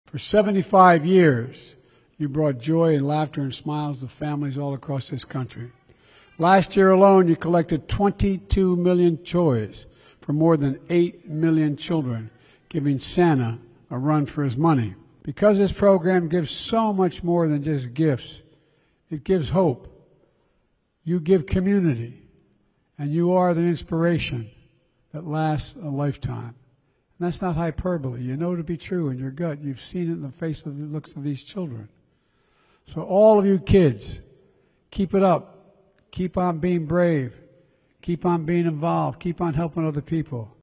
President Biden speaks at Toys for Tots event in Virginia
President Biden and First Lady Jill Biden participated in a United States Marine Corps Reserve sorting event for Toys for Tots on December 12, 2022. According to the White House, Toys for Tots has sent out 627 million toys to more than 281 million children since 1947.